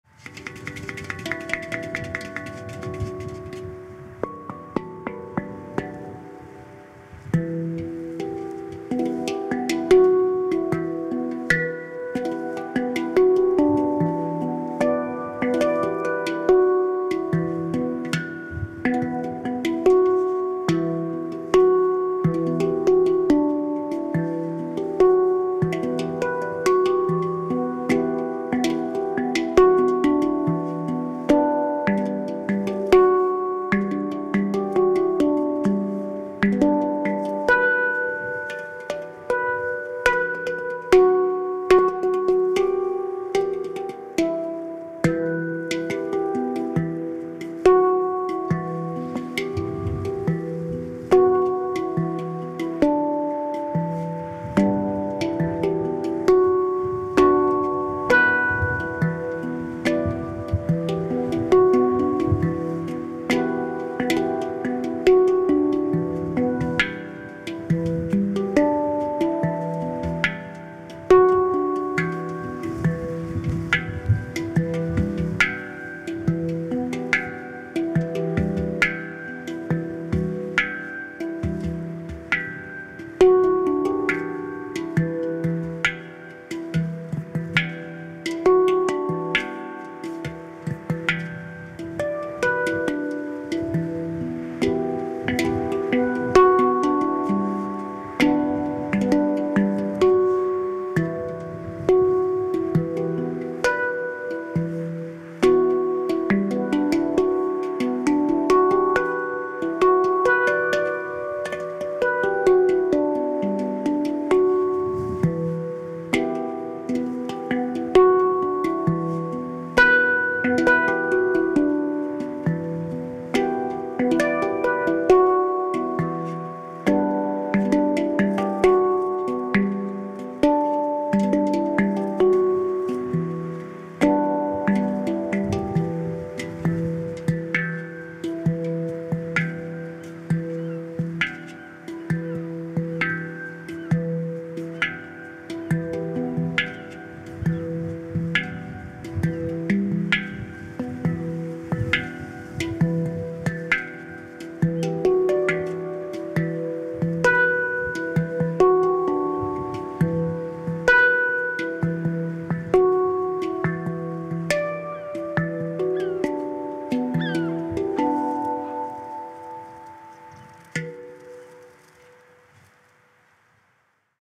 HANDPAN SESSION @ Cap Fagnet (Fécamp)
L'hiver dernier je suis allé me balader au Cap Fagnet à Fécamp avec mon handpan.

Enregistreur : Zoom F6
Microphone : Aston Element
Perche : Neewer NW-088
Casque : Beyerdynamic DT-770 Pro Black Edition

🛒 Visite ma boutique Amazon
📌 Tu as besoin de sons gratuits pour du sound design ?

📝 Abonnés à la sortie : 4 701.